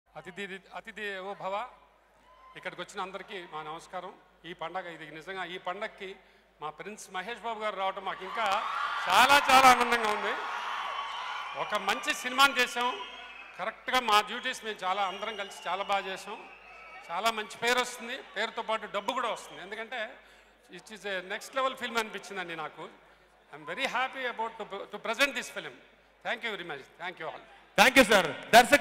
Producer Sivalenka Krishna Prasad Speech @ Sammohanam Pre-Release Event | Sudheer Babu
Watch & Enjoy Producer Sivalenka Krishna Prasad Speech Sammohanam Pre-Release Event. Starring SudheerBabu and AditiRaoHydari. Directed by MohanakrishnaIndraganti and Music by VivekSagar. Produced by Sivalenka Krishna Prasad on Sridevi Movies banner.

Movie : Sammohanam
Banner : Sridevi Movies
Producer : Sivalenka Krishna Prasad
Director : Mohanakrishna Indraganti
Music Director : Vivek Sagar
Cast : Sudheer Babu, Aditi Rao Hydari
Editor : Marthand K Venkatesh
Director of Photography : PG Vinda
Music Label: Aditya Music